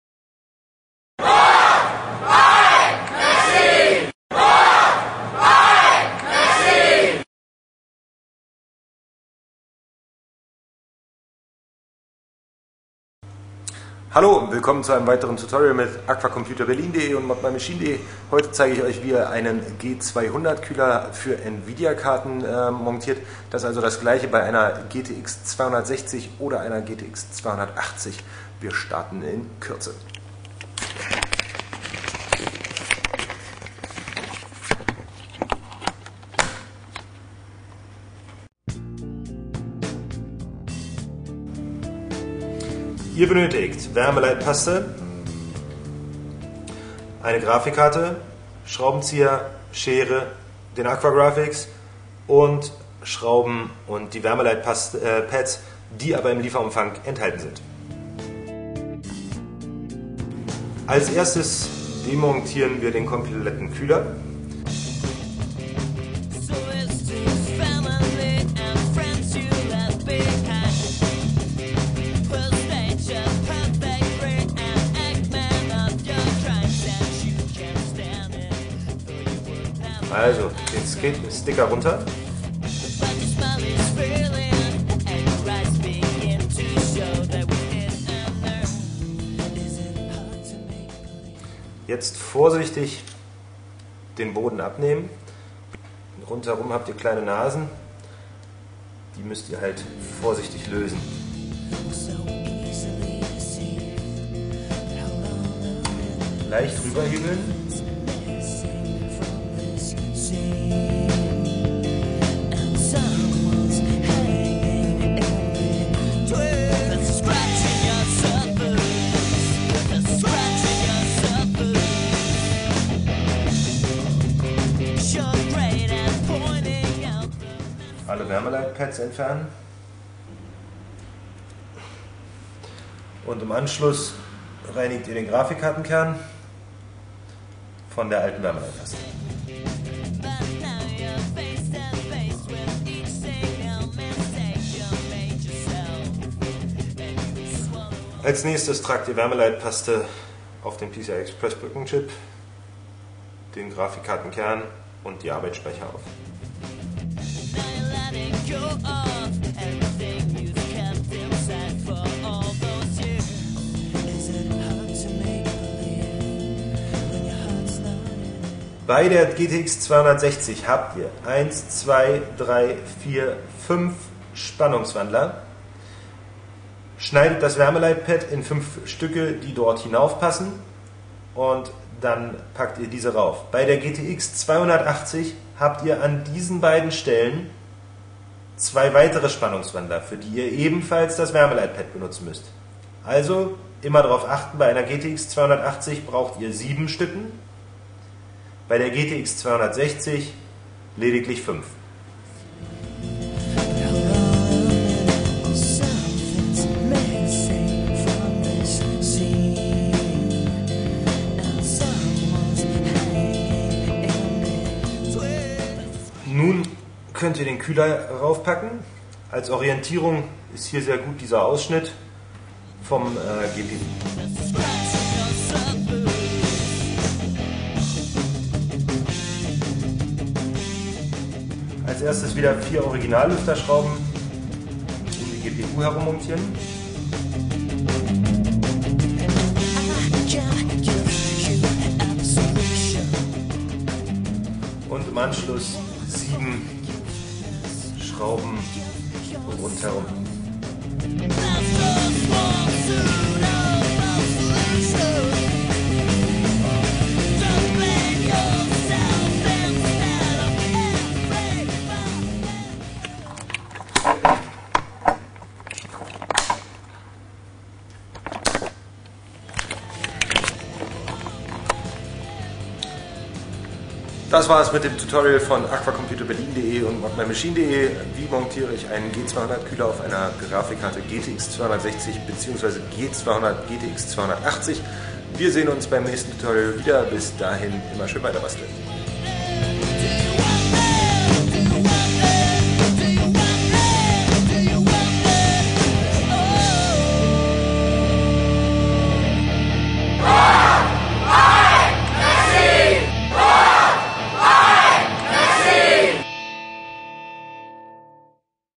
Wasserkühlung GTX 260 + GTX 280 Installation
In Zukunft wird es für jeden Grafikkartenkühler auch das passende Video geben, da es immer wieder Unterschiede gibt. Hier nun das Tutorial für NVidia GTX 260 und GTX 280 Grafikkarten.